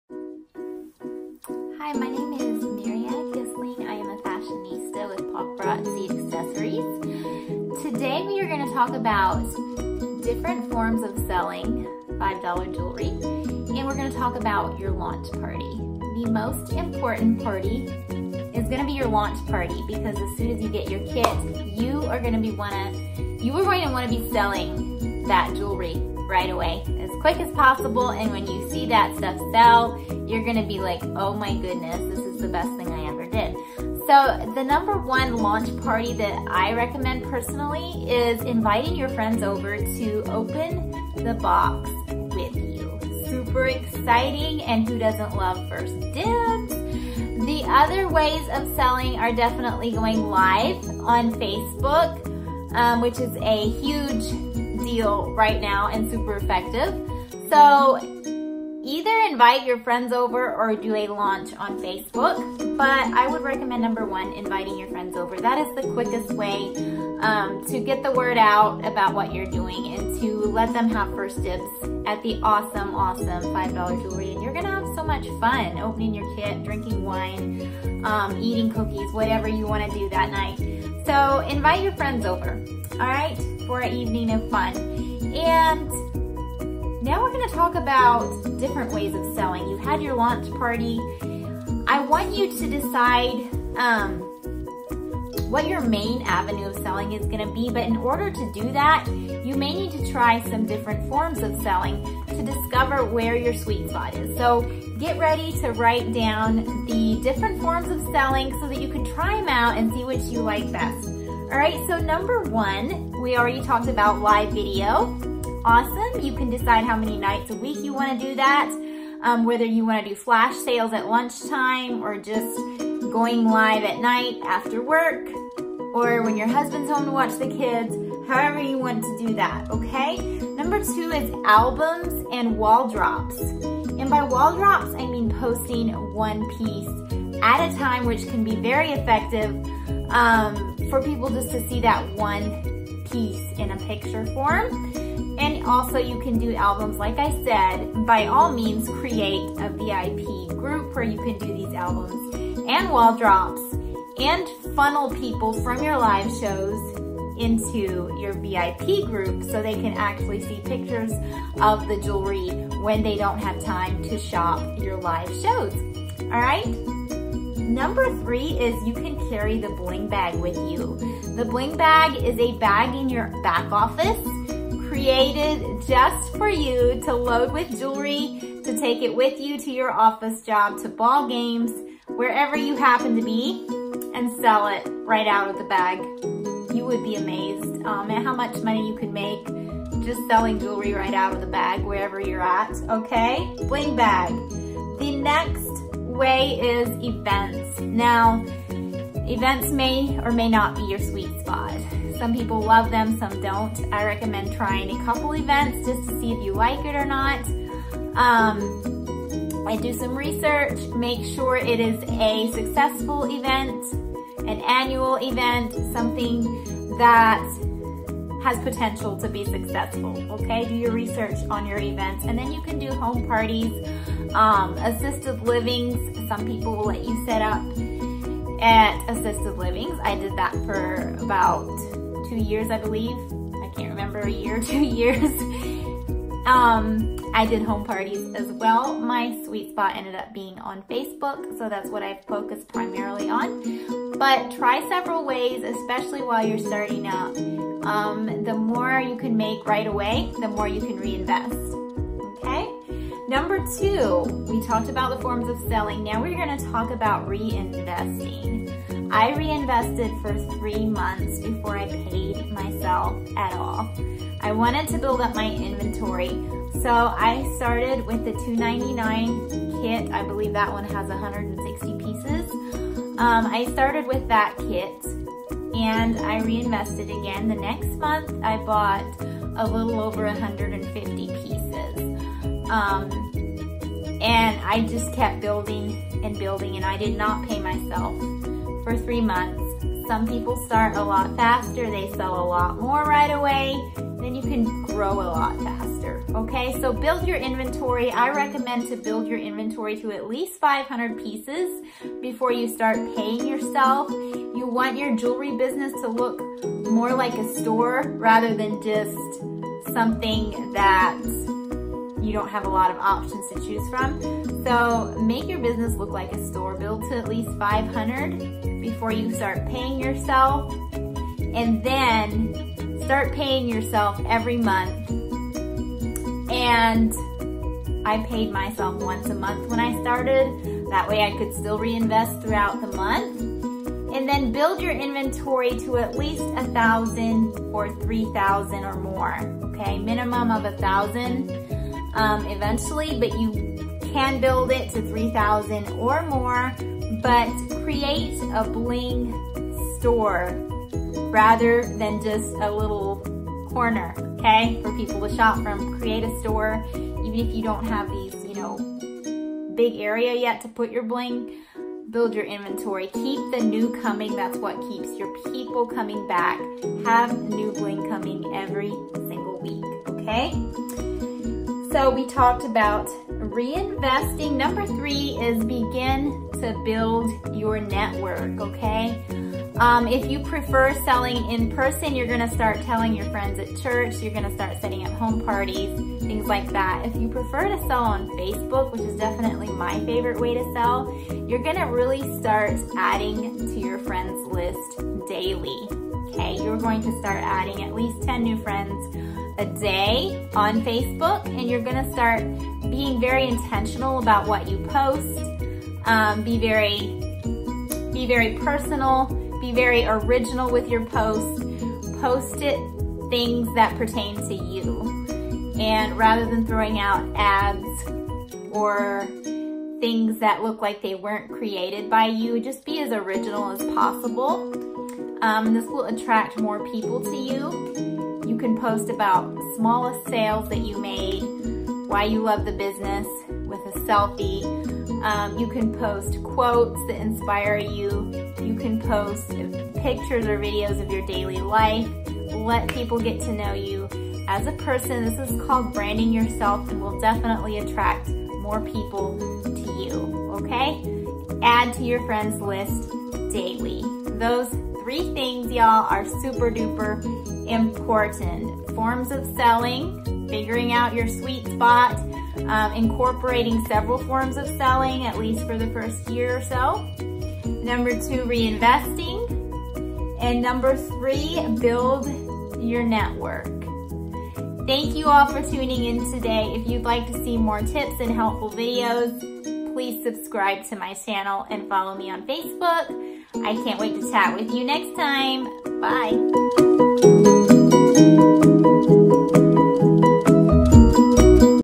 How To Sell Your Paparazzi Jewelry
All about different forms of making money with Paparazzi and How to Reinvest.